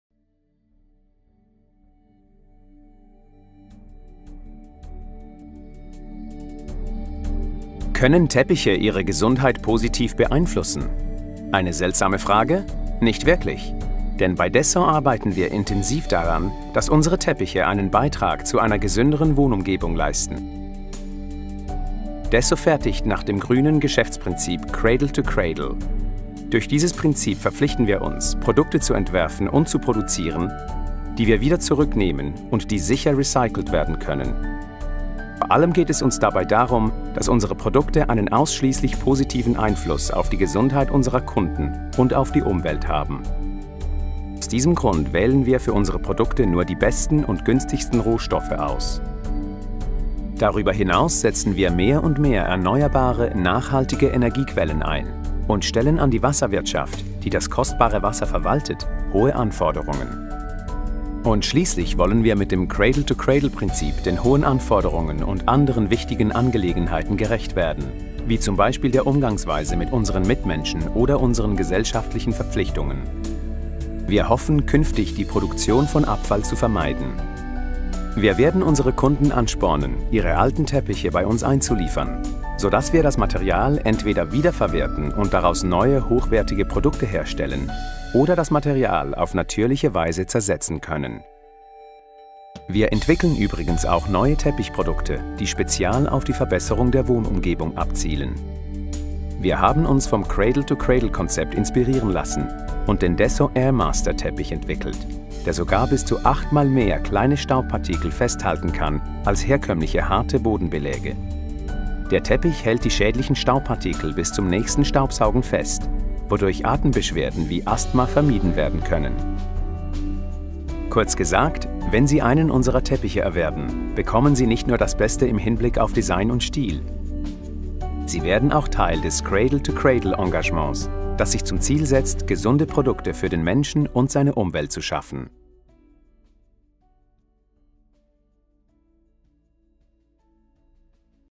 Desso Consumer Carpet- Cradle to Cradle German version
Desso fertigt nach dem grünen Geschäftsprinzip Cradle to Cradle®. Durch dieses Prinzip verpflichten wir uns, Produkte zu entwerfen und zu produzieren, die wir wieder zurücknehmen und die sicher recycelt werden können. Vor allem geht es uns dabei darum, dass unsere Produkte einen ausschließlich positiven Einfluss auf die Gesundheit unserer Kunden und auf die Umwelt haben. Aus diesem Grund wählen wir für unsere Produkte nur die besten und günstigsten Rohstoffe aus. Darüber hinaus setzen wir mehr und mehr erneuerbare, nachhaltige Energiequellen ein und stellen an die Wasserwirtschaft, die das kostbare Wasser verwaltet, hohe Anforderungen.